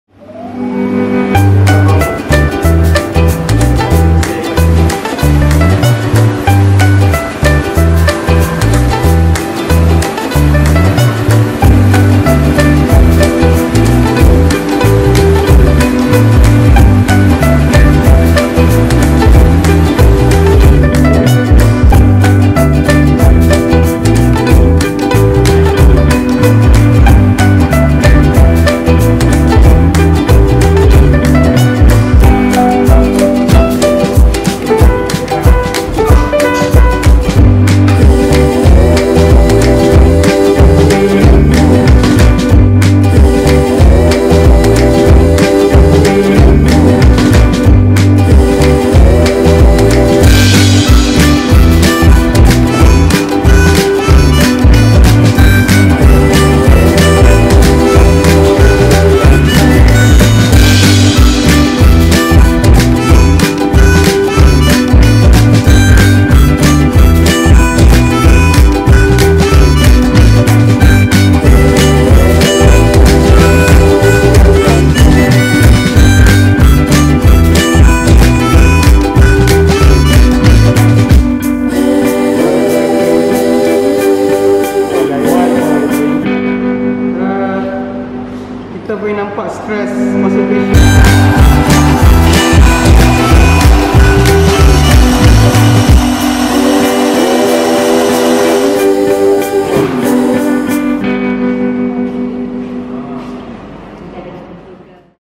Additive Manufacturing SLM of a brake caliper
Additive Manufacturing using a SLM machine to manufacture a brake caliper